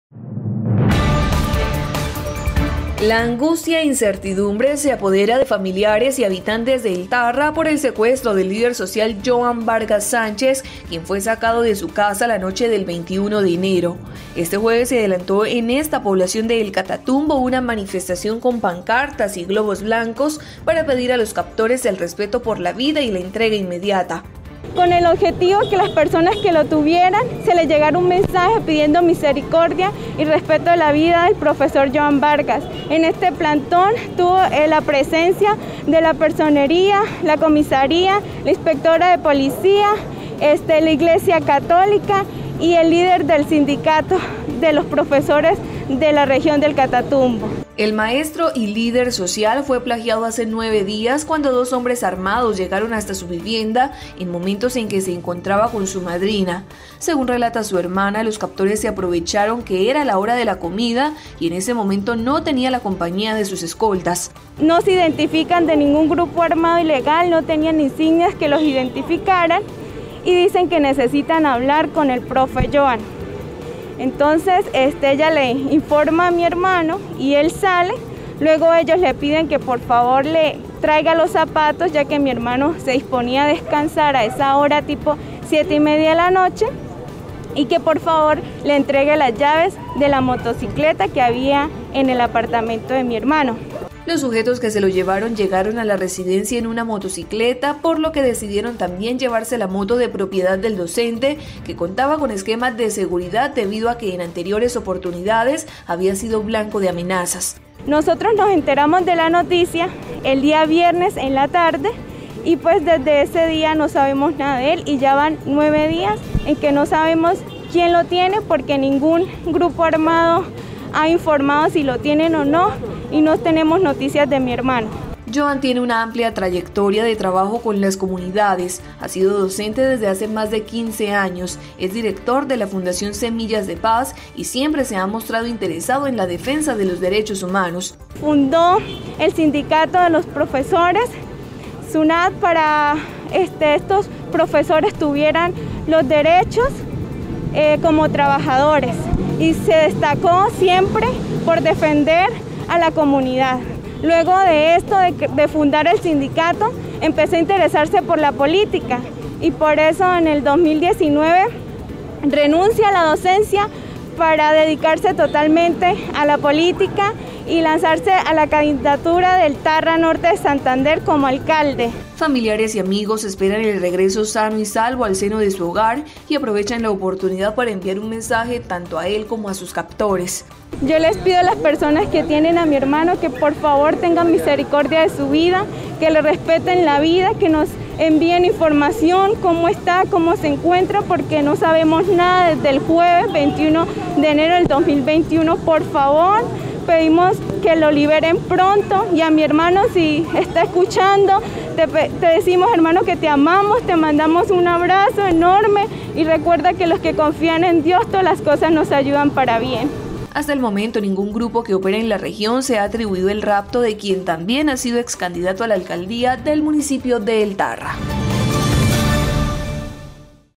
EN EL TARRA SE ADELANTÓ MANIFESTACIÓN POR LA LIBERTAD DE LÍDER SOCIAL SECUESTRADO
En el Tarra se adelantó una manifestación para pedir por la libertad de líder social secuestrado. Desde el 21 de enero no se conocen noticias de Johan Vargas, lo que aumenta la angustia de sus seres queridos.